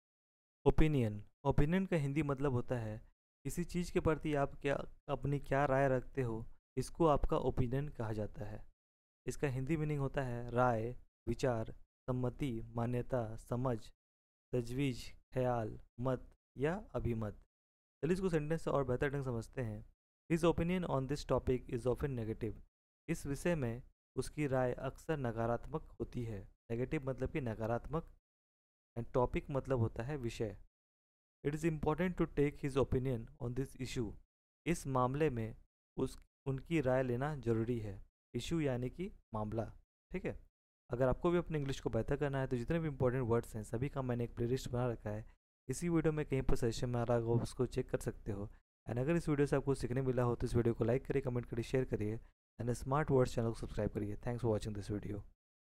Opinion Meaning In Hindi | Opinion Ka Matlab Kya Hota Hai | Opinion Definition in Hindi
Opinion Explanation in Hindi
Check More
[Hindi meaning]
राय, विचार, सम्मति, मान्यता, समझ, तजवीज़, ख़याल, मत, अभिमत
 [sentence example]
His opinion on this topic is often negative.
इस विषय पर उनकी राय अक्सर नकारात्मक होती है।
It is important to take his opinion on this issue.
इस मामले में उनकी राय लेना जरूरी है।

Recommended Book
Word Power Made Easy